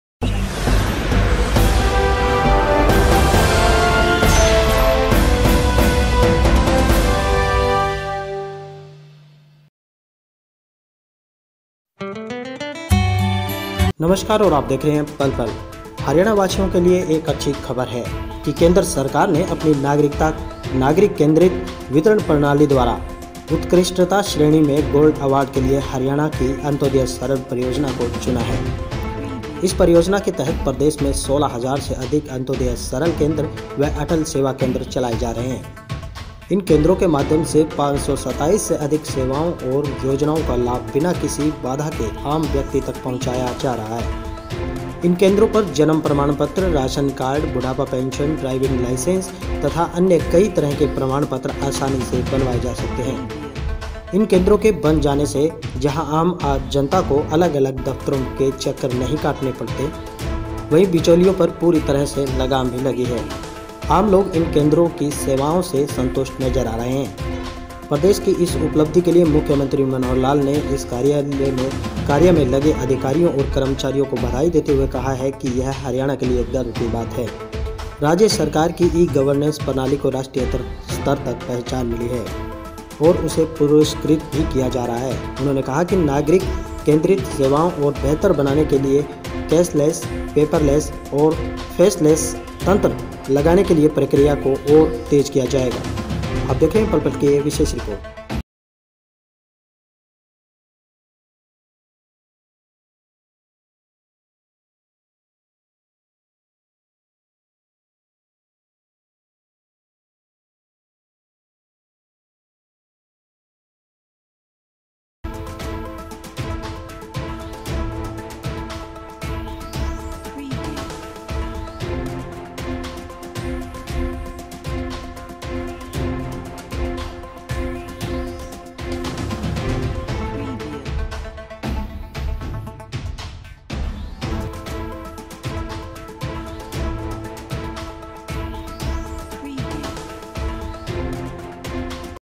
की ई-गवर्नेंस प्रणाली को मिलेगा गोल्ड अवार्ड।
आम नागरिकों को समयबद्ध और आसान सेवाएं देने में हरियाणा सरकार अव्वल रही है यही कारण है कि हरियाणा की अंत्योदय सरल परियोजना को  केंद्र सरकार ने ‘नागरिक-केंद्रित वितरण प्रदान करने में उत्कृष्टता’ की श्रेणी में ‘गोल्ड अवार्ड’ के लिए चुना है। ये अवार्ड भारत सरकार द्वारा ई-गवर्नेंस के लिए आयोजित होने वाले  23वें राष्ट्रीय पुरस्कार समारोह में 7 -8 फरवरी को प्रदान किया जाएगा।